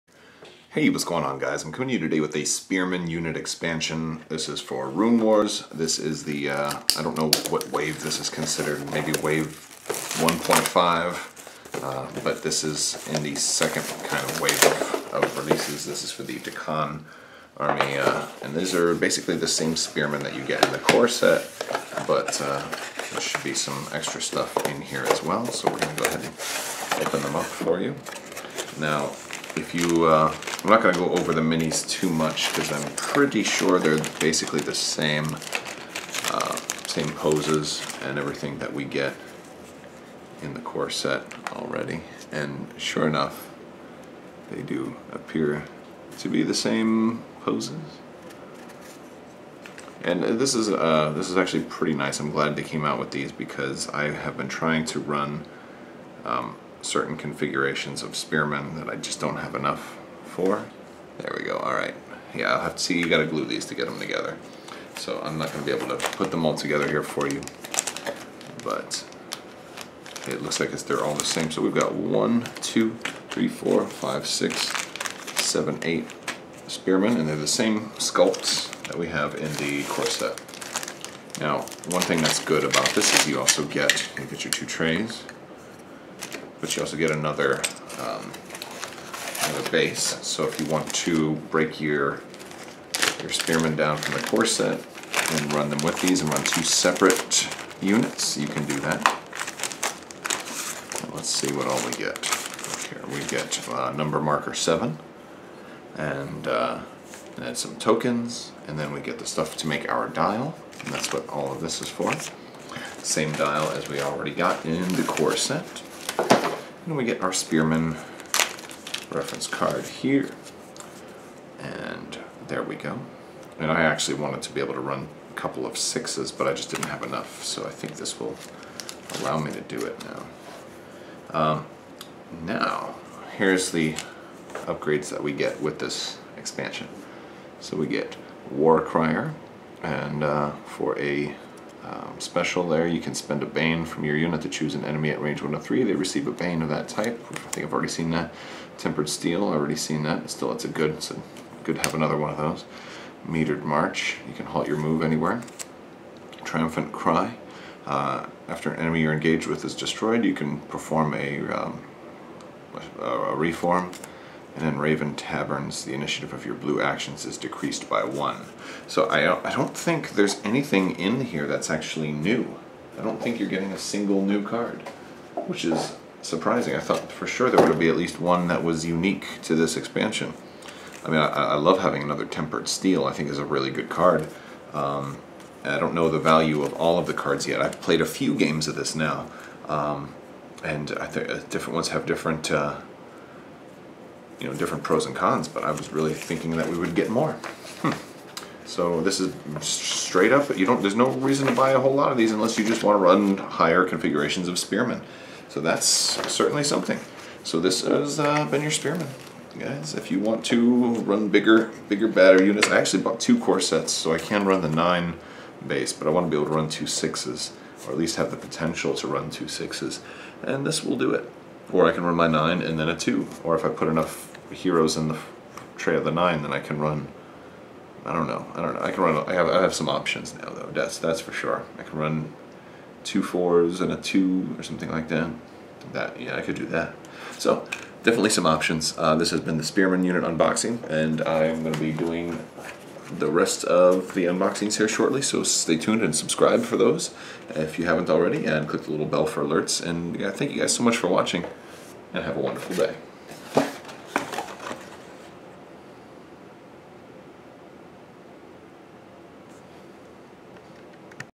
Runewars Spearmen Expansion Unboxing
Unboxing the Spearmen Expansion for Runewars Miniatures Game.